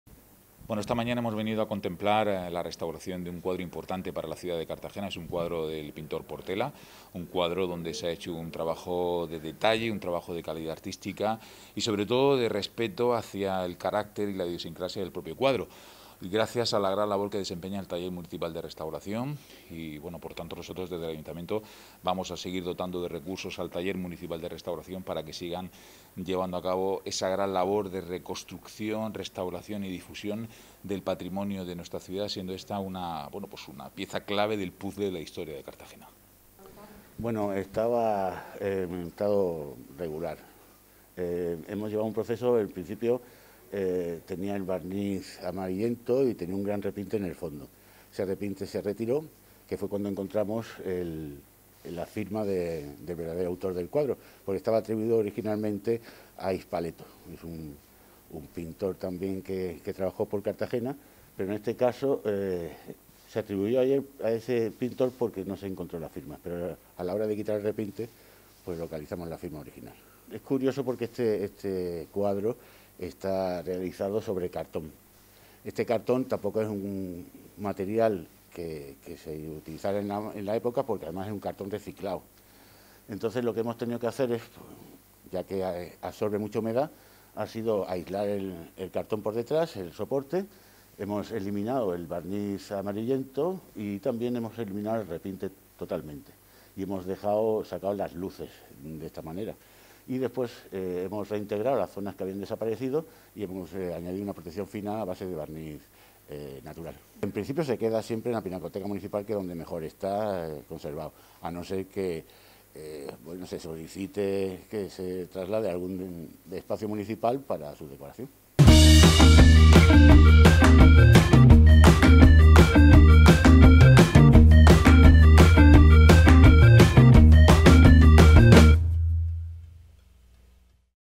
El Taller Municipal de Restauración descubre un cuadro del pintor Portela atribuido a otro artista
El Taller Municipal de Restauración ha descubierto entre los fondos municipales un cuadro del pintor Portela que, desde hacía más de cinco décadas, había sido atribuido al pintor Rafael García de la Espada (Hispaleto).

Este descubrimiento se ha producido durante la retirada de un estrato de color no original de la obra, denominada ‘Reposo’ y que representa el retrato de una mujer desconocida.

Así, tal y como explicaba Isidro Pérez, técnico de Restauración del Ayuntamiento, en este proceso, que ha durado aproximadamente un mes, se pudo recuperar la firma del autor que permanecía intacta y, tras un estudio comparativo con otras obras realizadas por Francisco Portela de la Llera pertenecientes a la Pinacoteca Municipal, se ha certificado su verdadera autoría.

Durante la presentación de la restauración de esta pintura, el Concejal Delegado de Cultura, Carlos Piñana, incidía en la necesidad de conservar nuestro pasado, ya que éste "nos enriquece como sociedad y como personas".  Según el edil, “el descubrimiento de un cuadro desconocido del Pintor Portela nos muestra la importancia de destinar recursos a la protección de nuestro patrimonio, ya que se trata de una pieza que ayuda a completar el puzzle que conforma la historia de Cartagena”.

Otra peculiaridad de este óleo sobre cartón es el origen del soporte, ya que se trata del reaprovechamiento de una caja que, según el sello que luce compuesto por la vara de Esculapio cruzada con un ancla, indica su posible pertenencia al cuerpo de Farmacia de Marina. Se convierte, pues, en ejemplo de reciclaje en el siglo pasado.

UN GADITANO CON ALMA CARTAGENERA

Francisco de Paula Portela de la Llera nació el 29 de noviembre de 1869 en Puerto Real (Cádiz), pero puede considerarse como cartagenero, porque desde los trece años reside ininterrumpidamente en la ciudad portuaria. Su padre era capitán de la marina mercante. Desde adolescente estudió dibujo en las clases de la Sociedad Económica de Amigos del País de Cartagena, de la que fue luego profesor. Falleció en Cartagena en 1950.

Si en Murcia Sánchez Picazo es el pintor de las flores, en Cartagena Portela es el pintor de la Marina de Guerra. Este ha sido, sin duda, su tema predilecto, y en sus cuadros ha captado la bella línea de los modernos navíos, como también la de las gloriosas escuadras de madera que sucumbieron heroicamente en Santiago y Cavite. A veces, y dentro del más estricto realismo, llevó también a su cuadros el tema de los astilleros, de la Albufera Valenciana y otros con veleros de paz.

Cuadro suyo, de tema no marino, es el de la "Coronación de la Virgen de la Caridad". A excepción de sus últimos años, toda la vida de Portela se dedicó a la enseñanza. Puede afirmarse que su estudio y el de Vicente Ros, eran las únicas academias de iniciación a la pintura que los jóvenes podían entrar en Cartagena. Portela preparaba para el ingreso en las academias militares. Ejerció como profesor de las Escuelas de Industrias (elemental y superior) y de la Escuela de Comercio.

Como artista expuso en Murcia, Cartagena y Madrid (Sala Atenea y salón de estampas del Museo de Arte Moderno). A la inauguración de estas exposiciones de Madrid asistieron altas autoridades de la Marina, así como representaciones de artistas de Valencia y Madrid. También expuso Portela, y ésta es la última de sus muestras, en la Delegación de Cultura de la Alta Comisaria de Marruecos en Tetuán en la que se le rindió homenaje al pintor, ofrecido por el almirante Bastarreche.

El Ayuntamiento de Melilla adquirió su cuadro "El convoy de la victoria", tema inicial de la lucha española.

En Cartagena hay cuadros de Portela, de tema no marino, en la Iglesia del Carmen "Los Cuatro Evangelistas" y la "Virgen del Perpetuo Socorro"; en la capilla de la Capitanía General del Departamento Marítimo, los "Cuatro santos de Cartagena"; y en el Parque de Artillería, los retratos de Daoíz y Velarde. También existen cuadros en los fondos de la Pinacoteca Municipal de Cartagena, así como otros muchos en otras entidades y en poder de particulares.